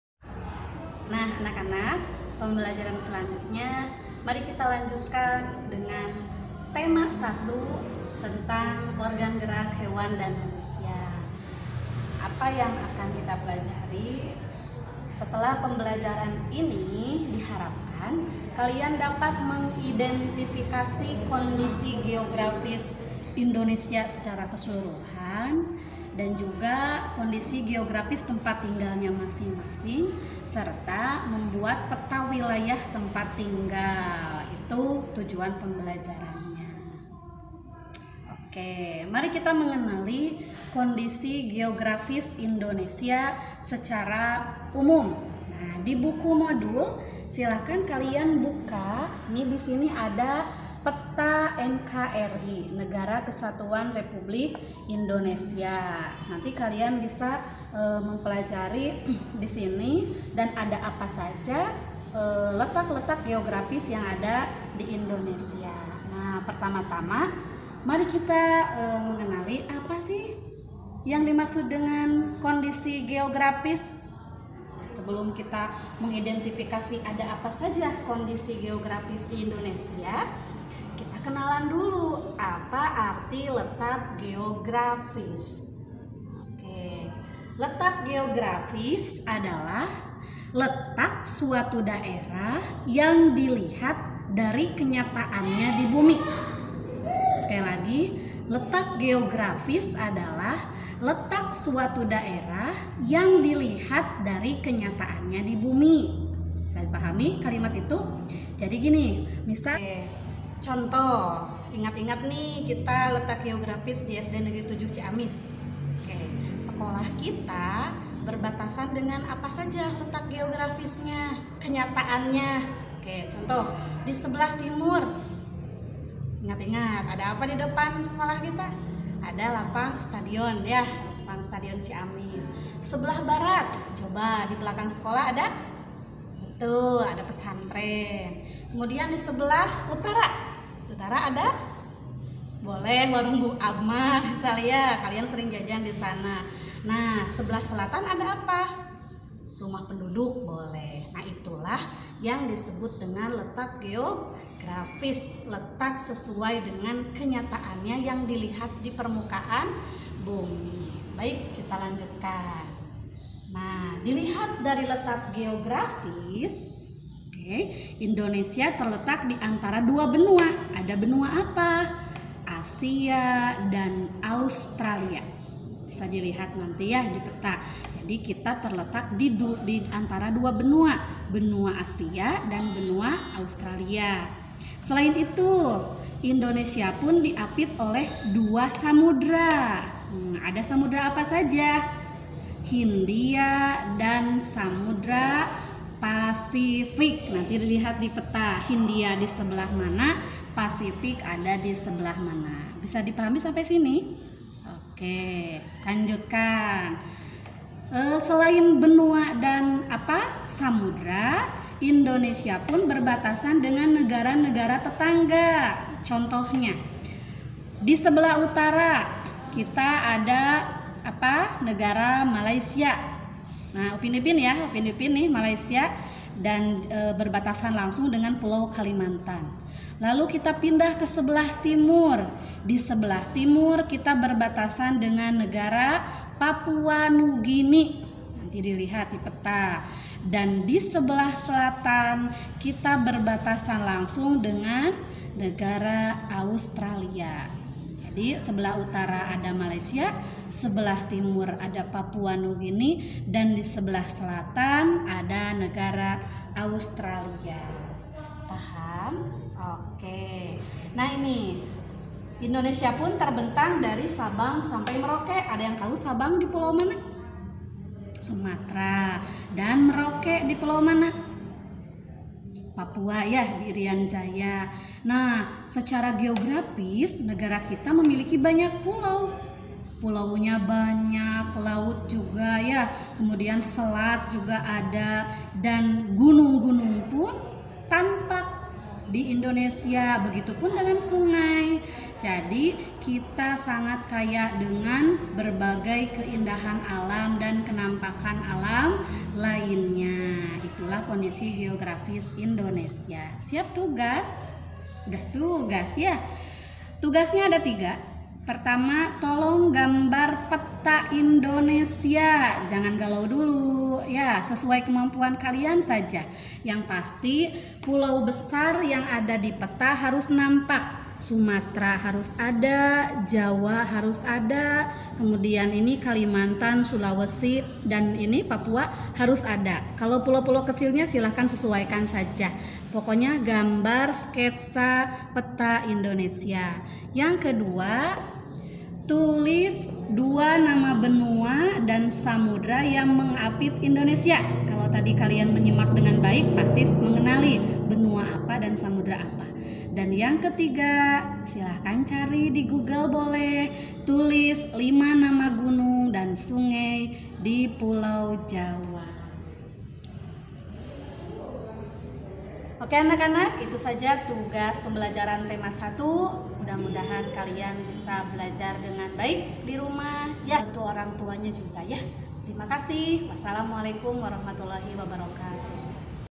Materi IPS Tema 1 Kelas 5 Daring Selama Covid 19
Video ini tentang materi IPS tema 1 kelas 5. Pelajari dengan seksama dan ceria. Selamat belajar